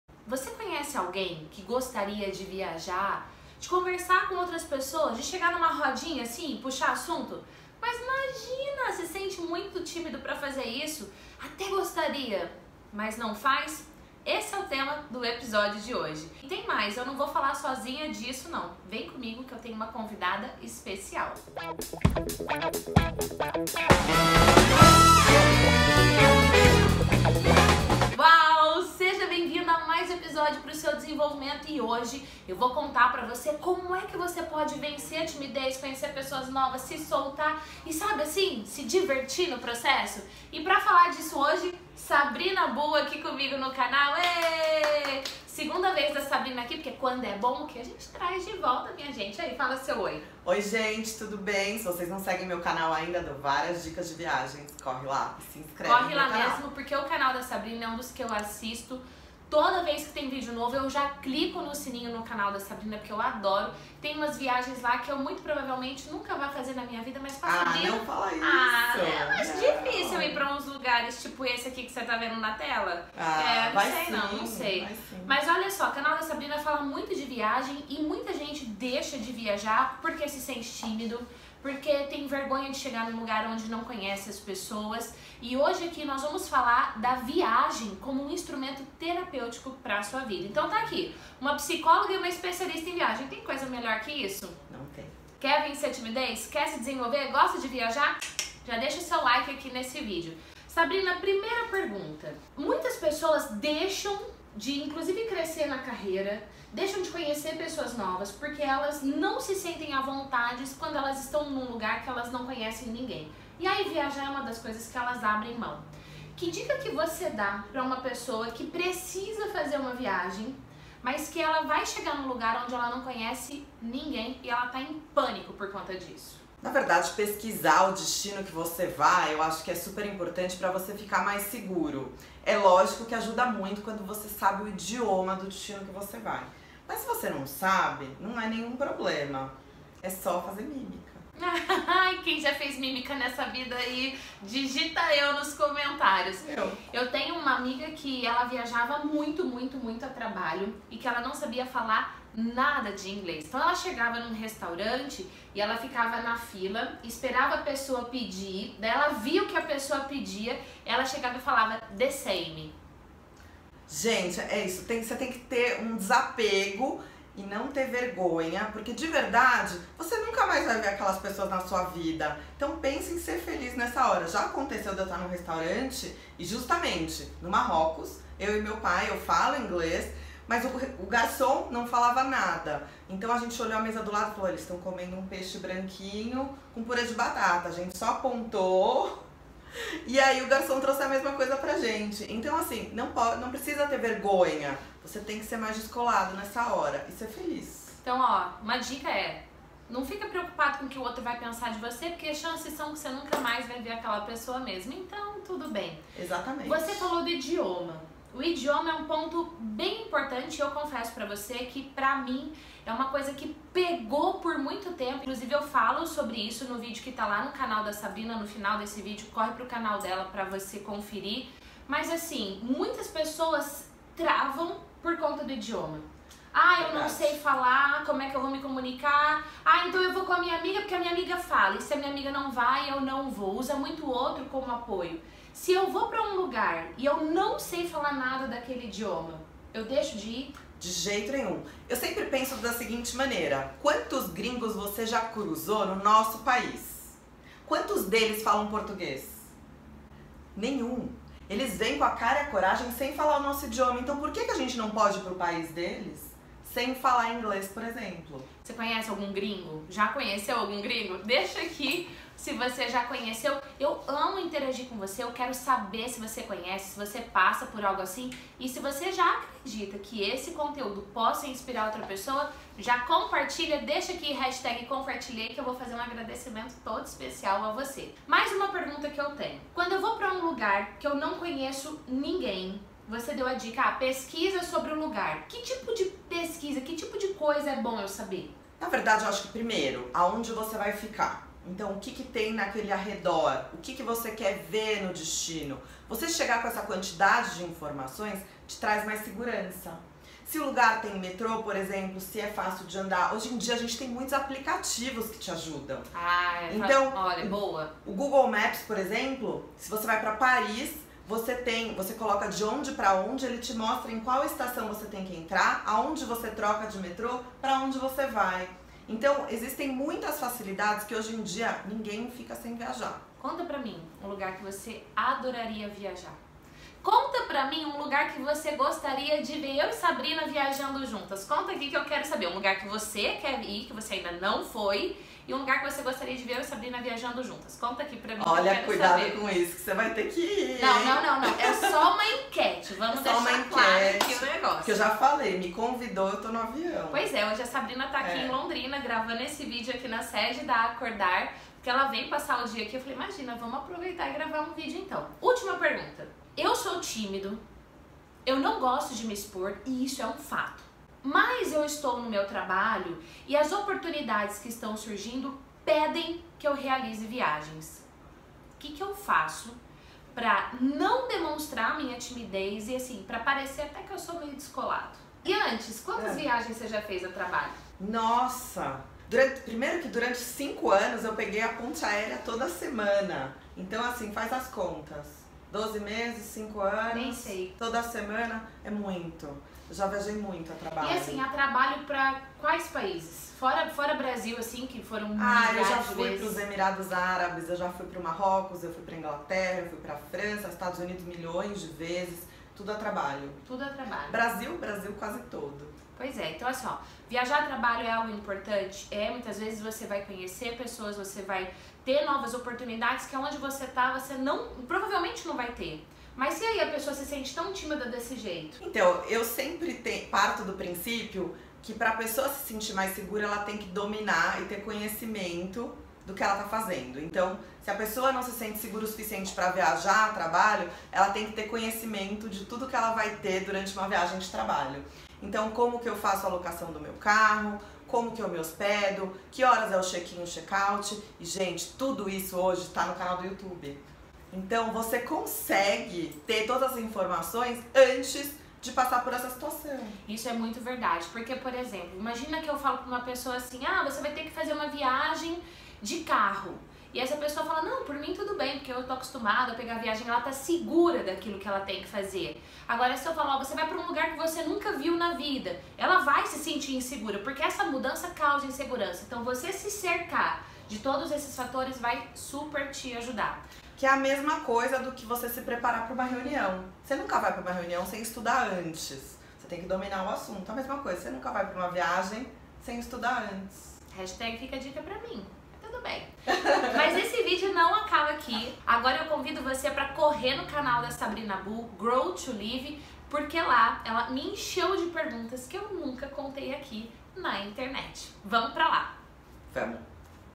Vencer a timidez... Como?
Vencer a timidez... Como? Nesta entrevista com Sabrina Bull você terá várias dicas para como vencer a timidez e vencer a vergonha!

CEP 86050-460